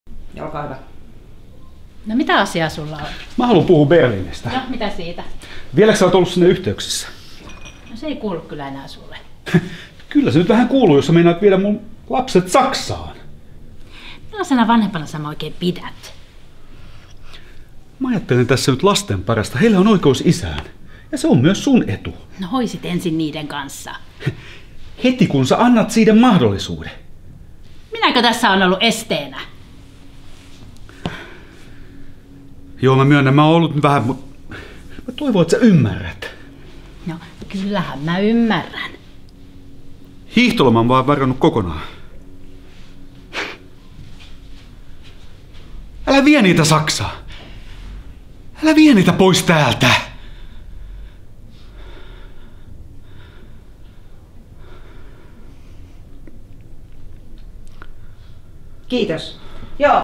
Dialogi 20.11.2022
Sarin kanssa tehty dialogi Mari Rantasilan kameranäytteleminen kurssilla. Dialogin teksti on Paavo Westerbergin Jälkilämmöstä.